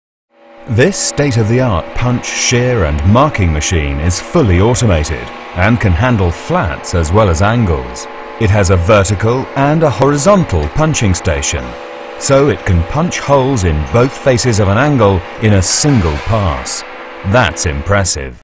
Pinch and Shear
Just a small piece of the fabrication equipment available at the EDS UK production facility.